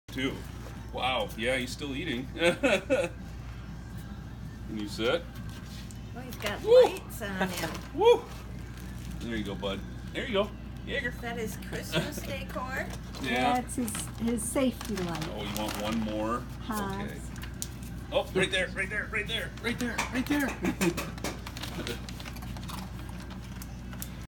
Hobbes Visits Hardware Store
Hobbes, the Wheaten Terrier, visits his favorite hardware store, Settergren's of Linden Hills for treats.